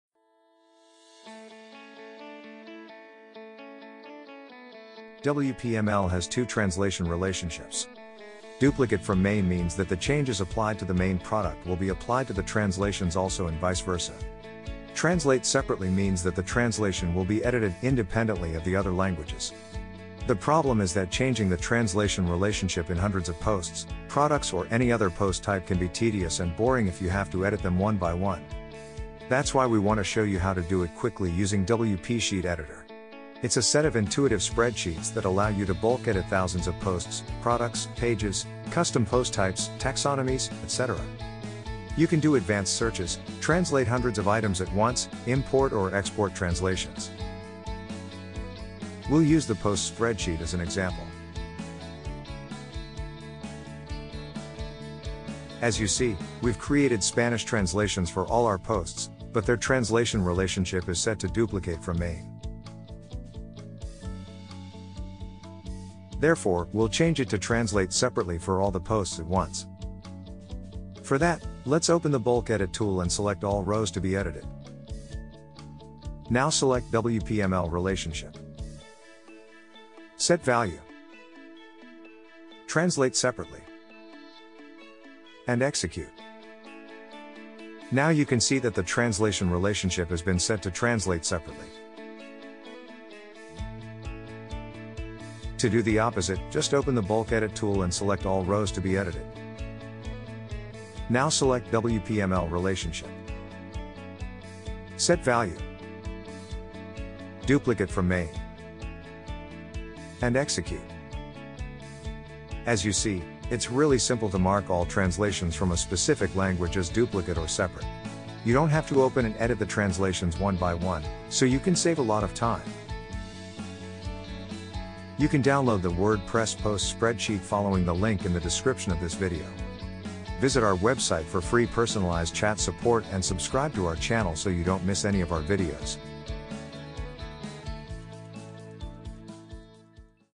WPML - How to Mark Translations as Duplicate or Separate
Learn how to mark all the WPML translations as duplicate or separate with a few clicks. Do it using WP Sheet Editor and save a lot of time!

This WordPress bulk editor allows you to save a lot of clicks because you can mark your WPML translations as duplicate or separate in bulk. You don't need to edit the WPML translations one by one.

Background Music